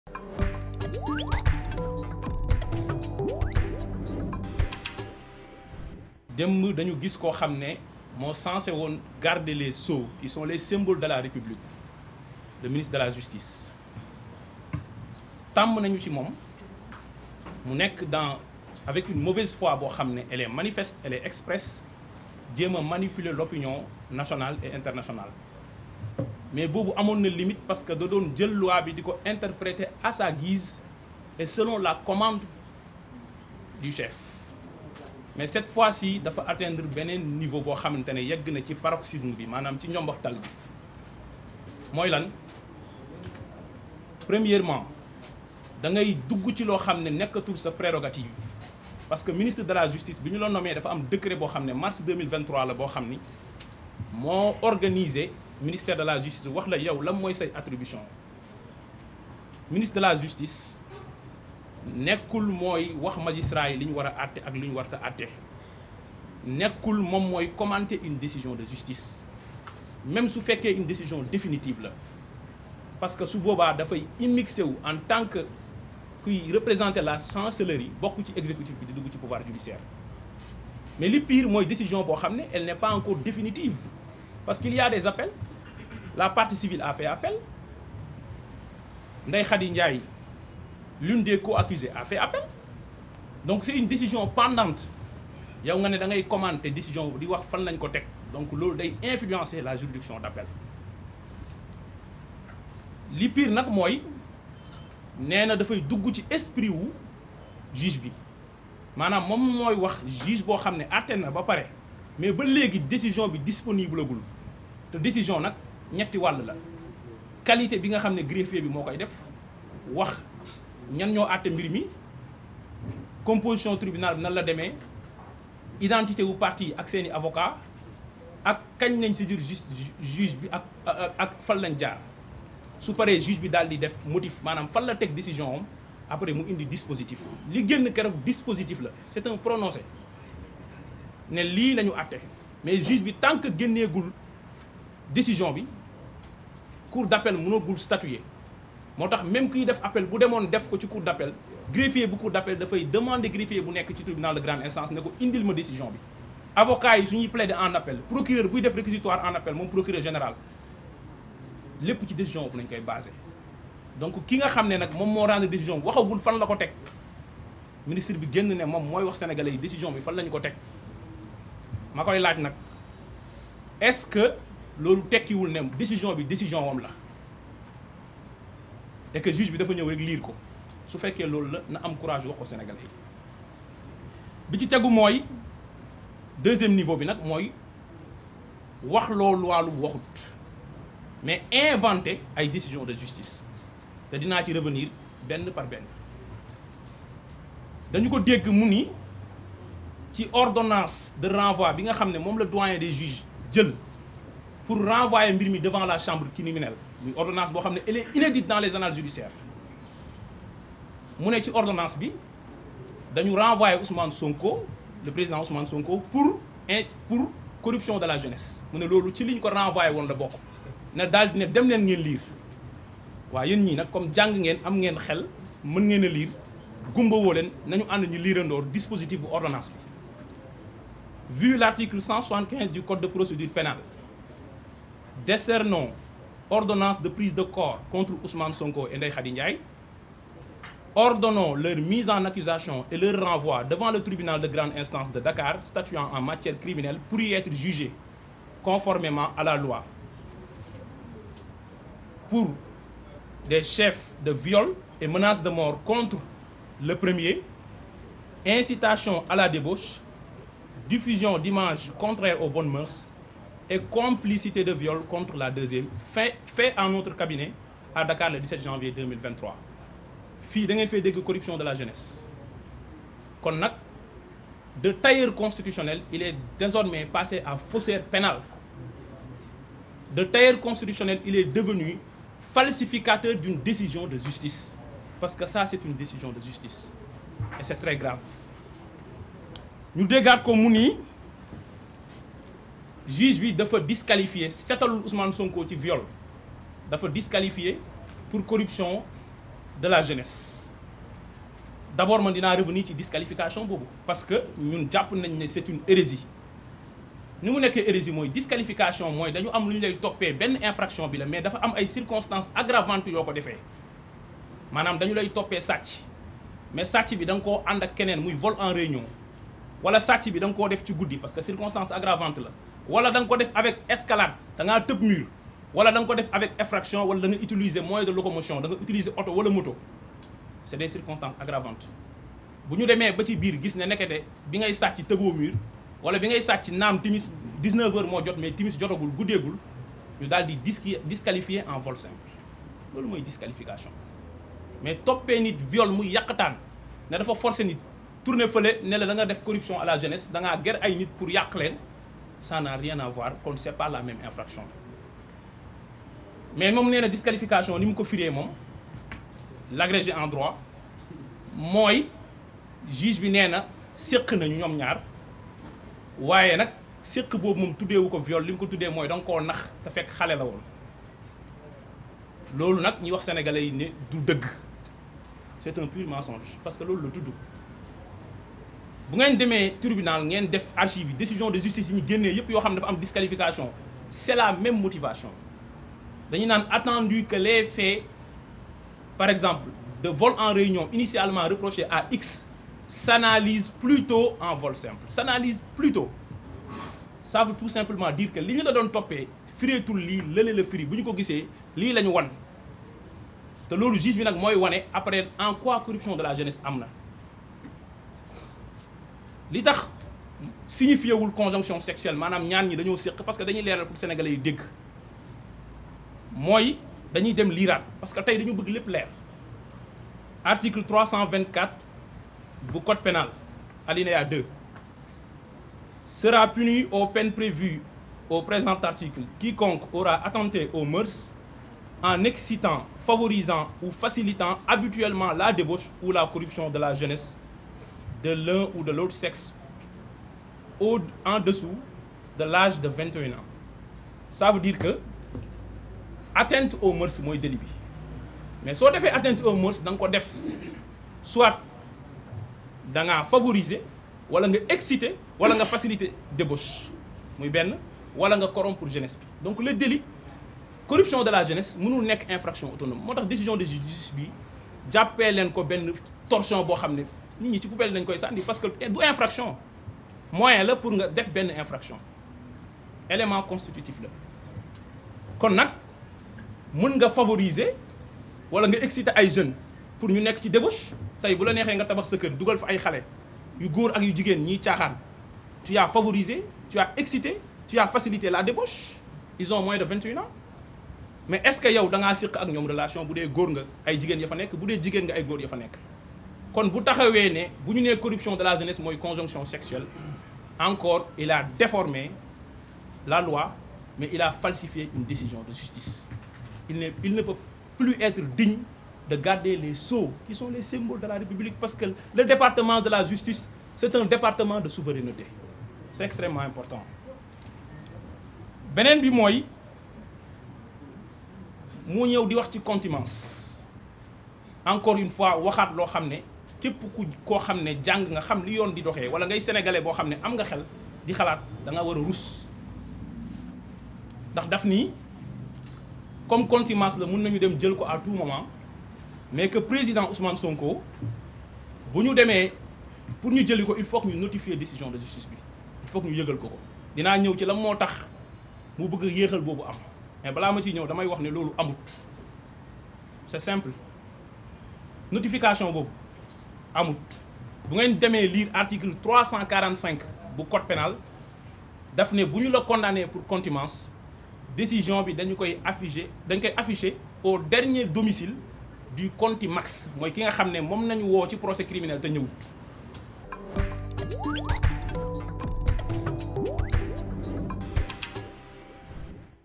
Condamnation d'Ousmane Sonko: Me Ngagne Demba Touré détruit Ismaela Madior Fall
Ngagne Demba Touré, membre de Pastef/Les patriotes et  de la coalition Yewwi Askan Wi a, face à la presse, indiqué que  « la mauvaise foi » du ministre de la Justice qui cherche à manipuler les populations, est manifeste. Le ministre de la Justice, dit-il, dépasse maintenant son attribution. Il ne lui revient pas de commenter une décision de justice.

 Cette approche d’Ismael Madior Fall démontre de l'immixtion de l'exécutif dans la justice. « Cela peut influencer la juridiction d'appel. Parce que, le dossier est toujours en cours. Il y a un prononcé tant que le juge ne sort pas la décision, la cour d'appel ne peut pas statuer. Le juge qui a prononcé la sentence n'a pas encore dit sur quelle base et le  ministre de la Justice se permet de le faire. Cela signifie tout simplement qu'il s'est immiscé dans le jugement. Ce qui catastrophique.

Le ministre de la Justice a affirmé que dans l'ordonnance de renvoi il figure la corruption de la jeunesse.   Ngagne Demba  dégage en touche et crie tout haut que ce délit ne figure pas dans l'ordonnance de renvoi devant la chambre criminelle d’Ousmane Sonko et Ndeye Khady Ndiaye. Pour lui, il s'agit d'un commentaire subjectif. " De tailleur de la constitution, Ousmane Sonko passe à falsificateur de décision de justice", regrette-t-il.

Le ministre de la Justice a aussi expliqué sur un plateau de télévision que le juge n'a pas acquitté Ousmane Sonko du crime de viol, mais a disqualifié  en corruption de la jeunesse. En réponse à cette assertion, le responsable politique de Pastef explique  qu'il s'agit d'une contrevérité. Pour lui, ce n'est pas la même infraction et il n’y a aucun link entre les deux chefs.